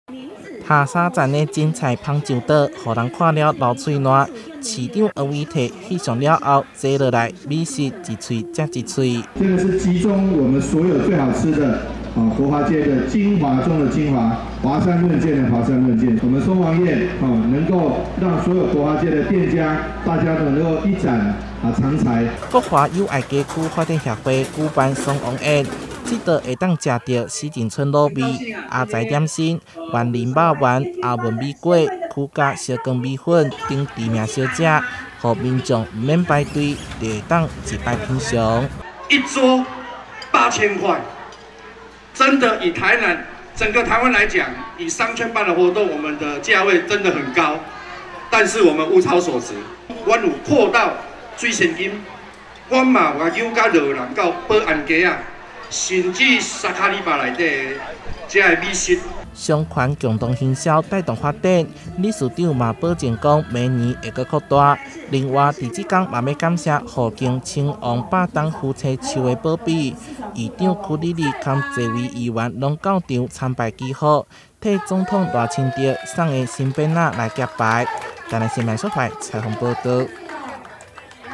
美食結合信仰｜國華松王宴登場 50間名店菜色一次品嚐
國華友愛街區發展協會 舉辦「松王宴」 這一桌可以吃到西井村滷味 阿財點心 員林肉圓 阿文米粿 邱家小卷米粉等知名小吃

台南國華街和友愛街巷仔內，有許多的特色美食，商圈特別結合在地自然信仰，推出「松王宴」的辦桌活動，超過50家店家、25道菜色的經典美食，讓不少民眾吃得相當過癮。